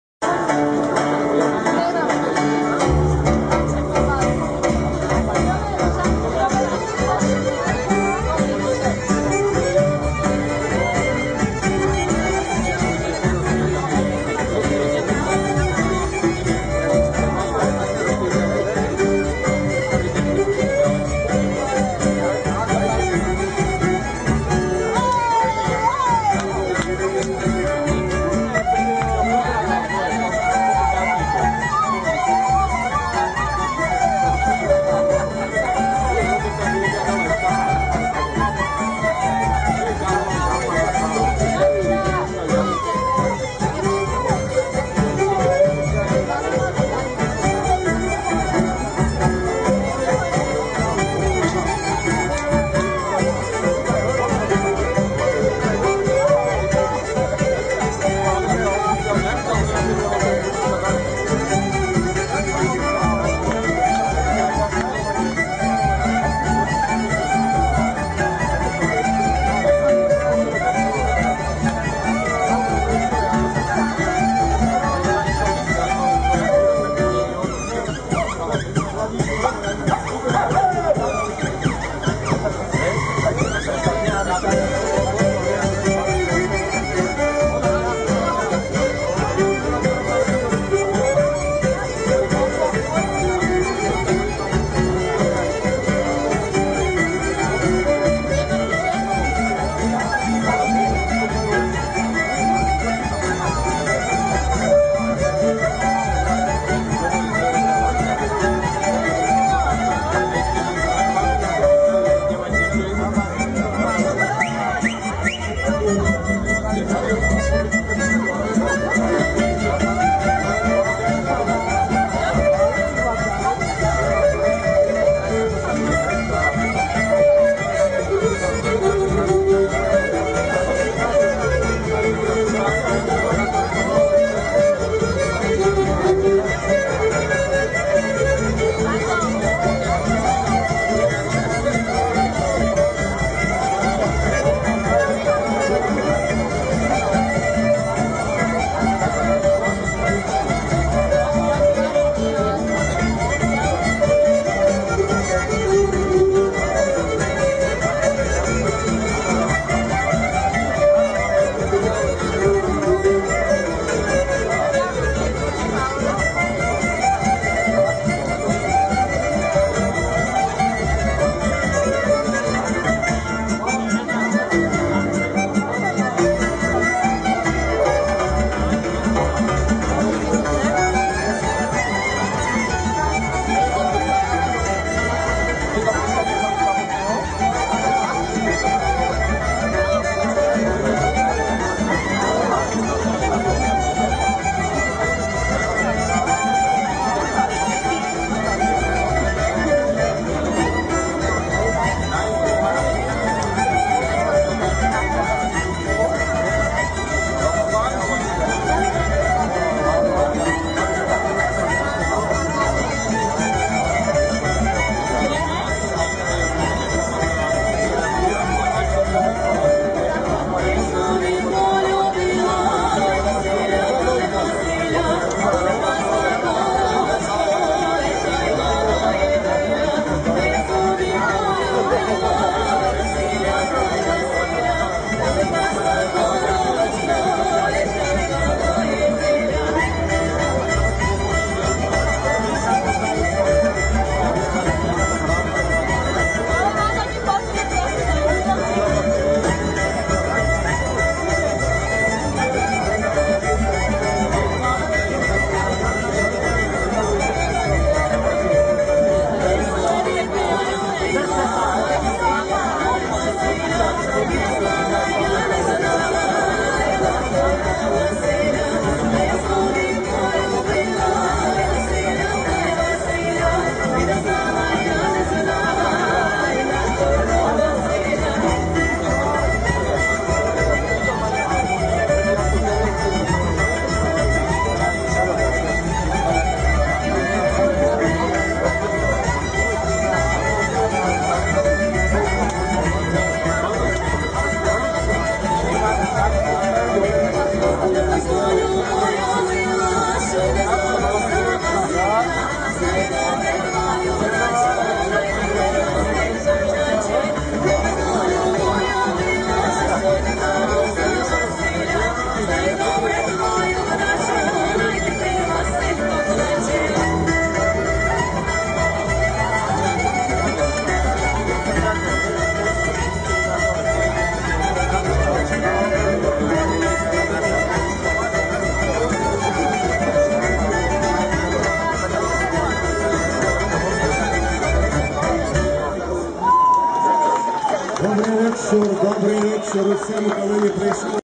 Hudaki Village Band - Jánošíkove dni 2011
Video z programu „Teraz vám vyhráva...", ktorý sa konal 30. júla 2011 v rámci Jánošíkových dní 2011.